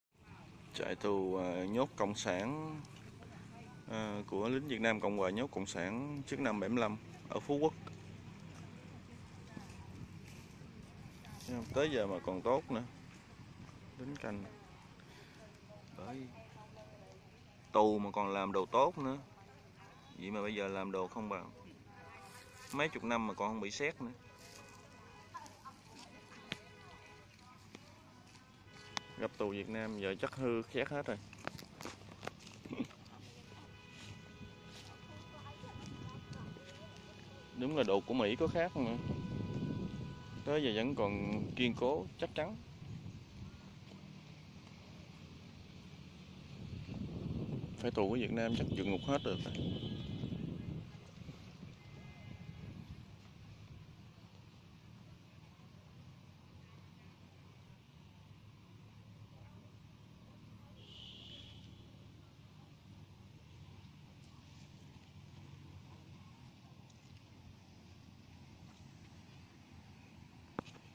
Phu Quoc prison
If you aren't aware of the islands history... Phú Quốc prison is a French built prison where political dissidents were detained and it was used for detention of captured Viet Cong and North Vietnamese soldiers during Vietnam war in the last century.

This peaceful tropical paradise, floats in the warm turquoise waters in the Gulf of Thailand, 50kms from the Vietnamese mainland and a 50 minute flight from Ho Chi Minh City (Saigon).